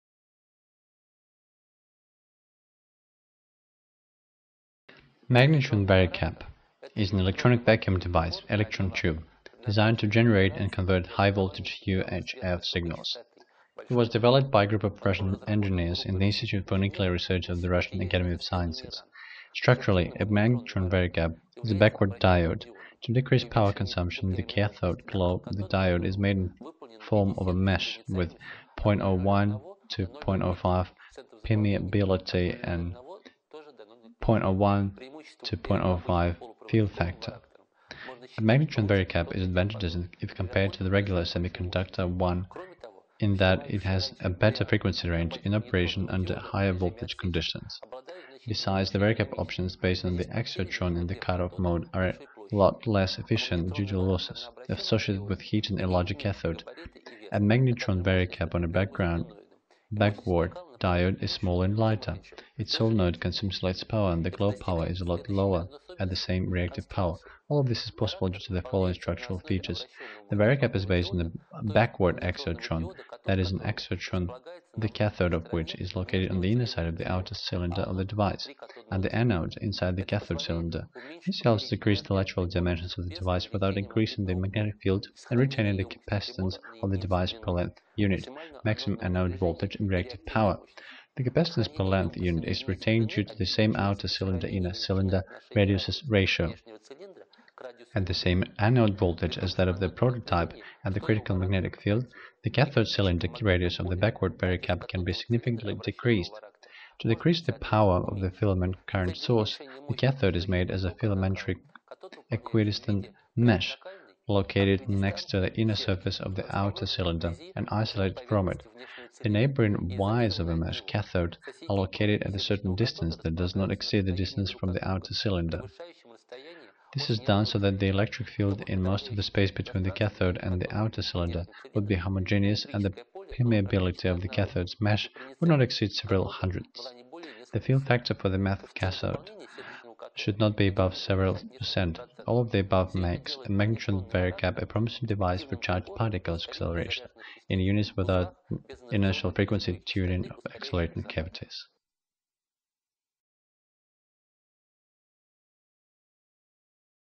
Magnetron Varicap
Magnetron Varicap        Magnetron varicap is an electronic vacuum device (electron tube) designed to generate and convert high-voltage UHF signals. It was developed by a group of Russian engineers in the Institute for Nuclear Research of the Russian Academy of Sciences.     Structurally, a magnetron varicap is a backward diode. To decrease power consumption the cathode glow of the diode is made in form of a mesh with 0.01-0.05 permeability and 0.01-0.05 fill factor. A magnetron varicap is advantageous if compared to the regular semiconductor one in that is has a better frequency range in operation under higher-voltage conditions.    Besides, the varicap options based on the axiotron in the cutoff mode are a lot less efficient, due to losses associated with heating a larger cathode. A magnetron varicap on a backward diode is smaller and lighter. Its solenoid consumes less power, and the glow power is a lot lower at the same reactive power.    All of this is possible due to the following structural features:    The varicap is based on a backward axiotron, that is, an axiotron the cathode of which is located on the inner side of the outer cylinder of the device, and the anode - inside the cathode cylinder. This helps decrease the lateral dimensions of the device without increasing the magnetic field and retaining the capacitance of the device per length unit, maximum anode voltage and reactive power.    The capacitance per length unit is retained due to the same outer cylinder-inner cylinder radiuses ratio. At the same anode voltage as that of the prototype, and the critical magnetic field, the cathode cylinder radius of the backward varicap can be significantly decreased.    To decrease the power of the filament current source, the cathode is made as a filamentary equidistant mesh located next to the inner surface of the outer cylinder and isolated from it.    The neighboring wires of a mesh cathode are located at a certain distance that does not exceed the distance from the outer cylinder. This is done so that the electric field in most of the space between the cathode and the outer cylinder would be homogenous and the permeability of the cathode's mesh would not exceed several hundredths.    The fill factor for the mess cathode should not be above several percent. All of the above makes a magnetron varicap a promising device for charged particles accelerator in units without inertial frequency tuning of accelerating cavities.